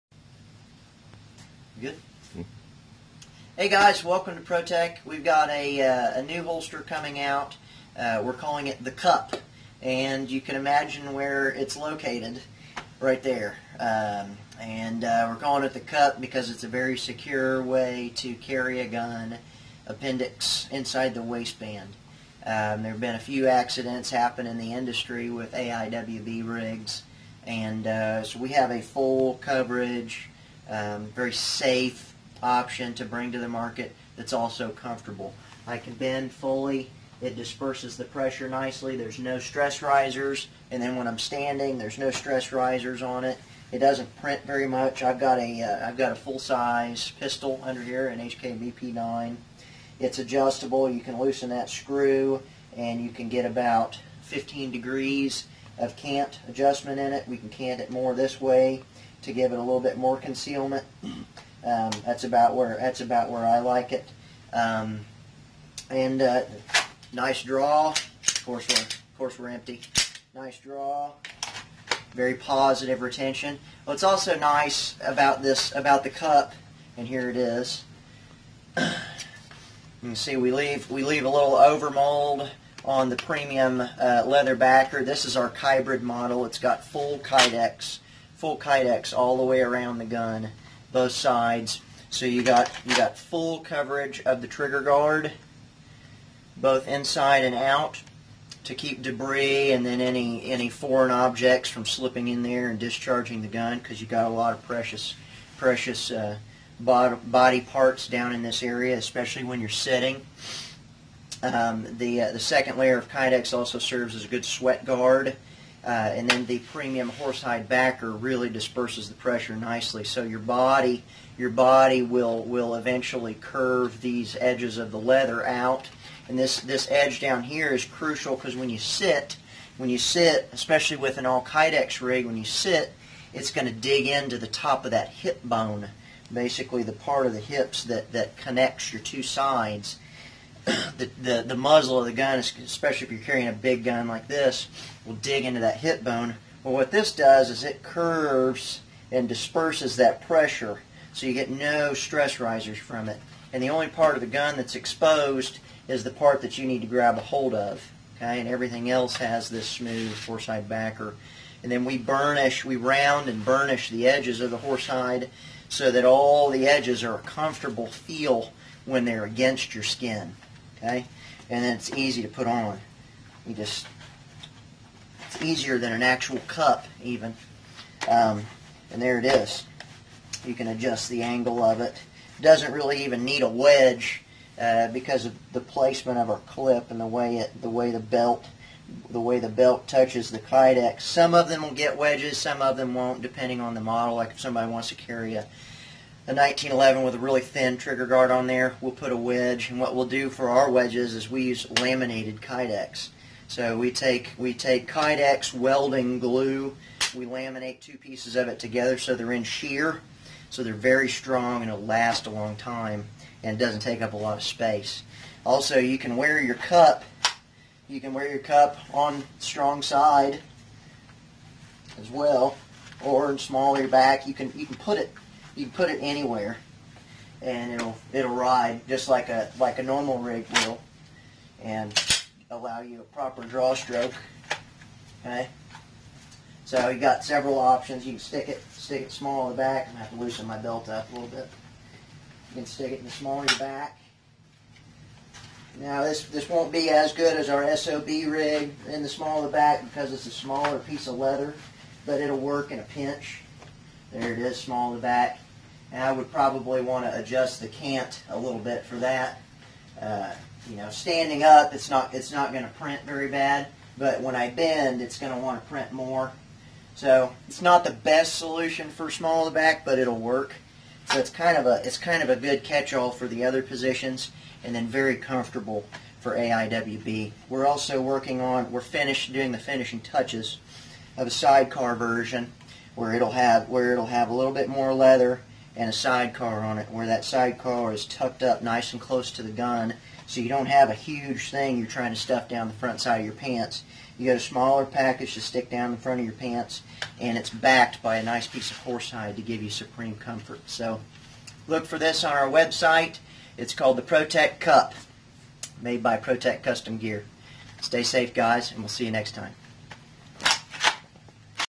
AIWB Holster, Comfortable and Safe! The Cup
The Cup is an AIWB Solution with comfort and safety and reliability at the forefront of its design.